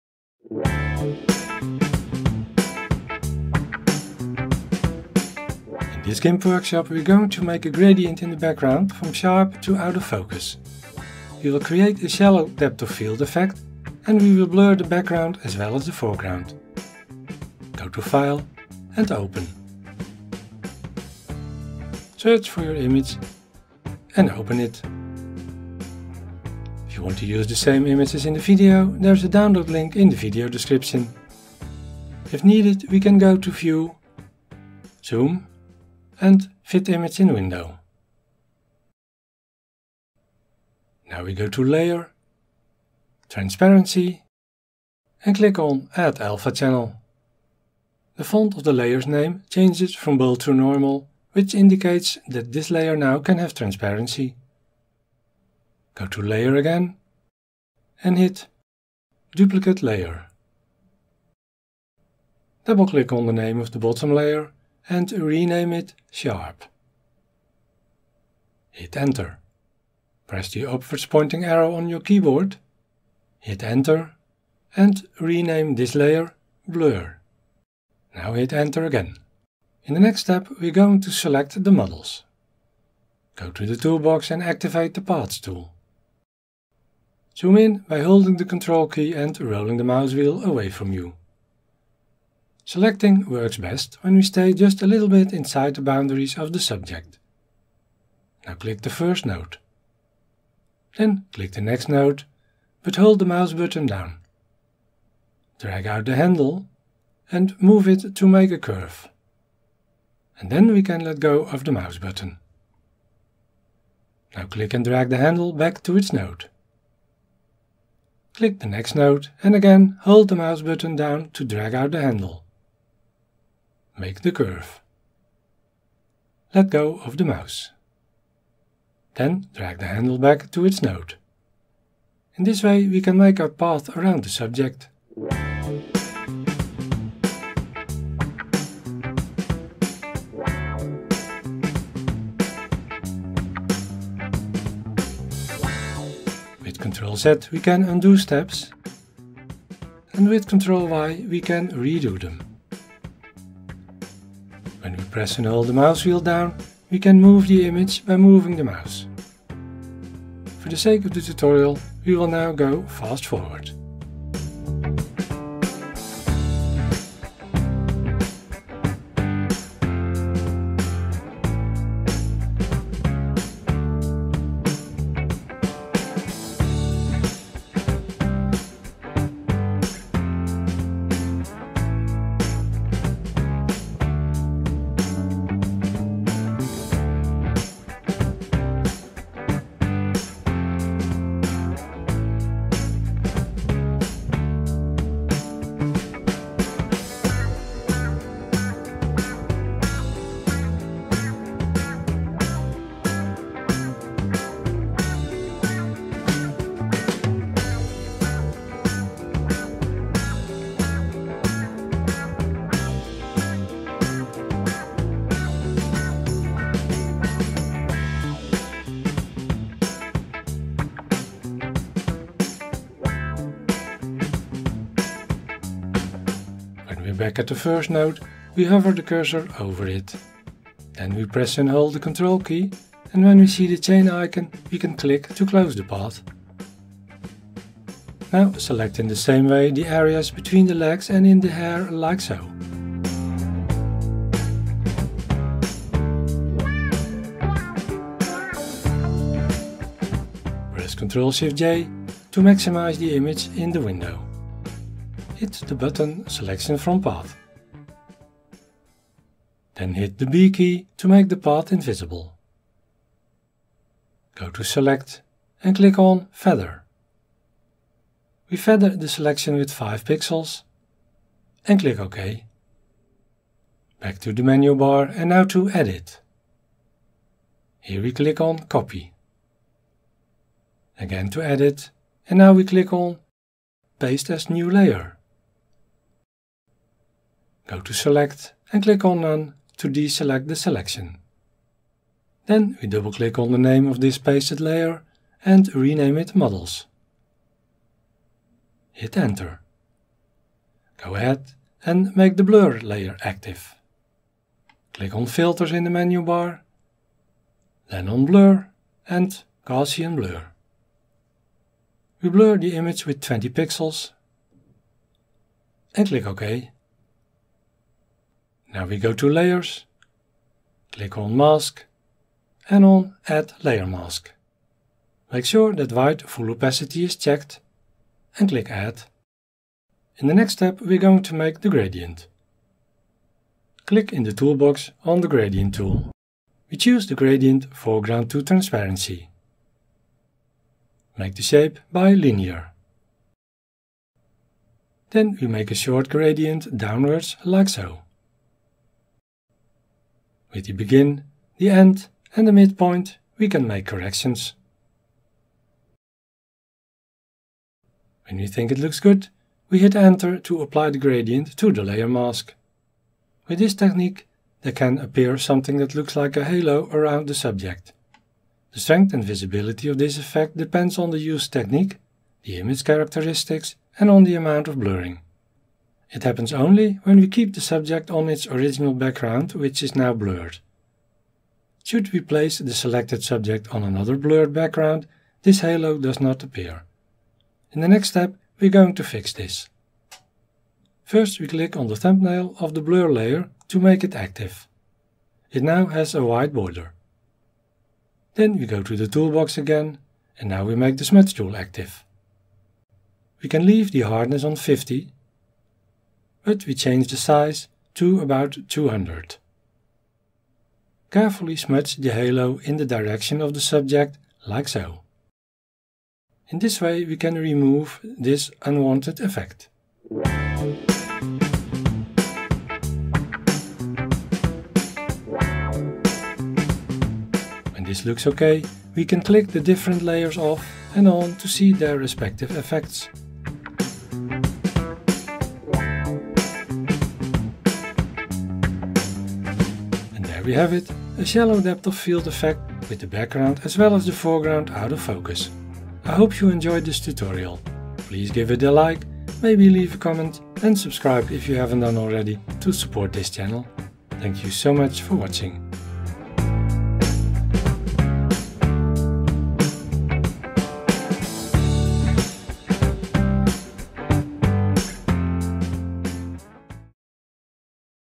Gimp: Blur Background And Foreground With Gradient Tool
In this tutorial we will see, how we can blur the background, as well as te foreground in an image, using the gradient tool and a layer mask.

Gimp 2.10.4 standard installation – Windows 10

Click on the CC button at the bottom of the video for English subtitles. Sorry for my 'home made' English 😉.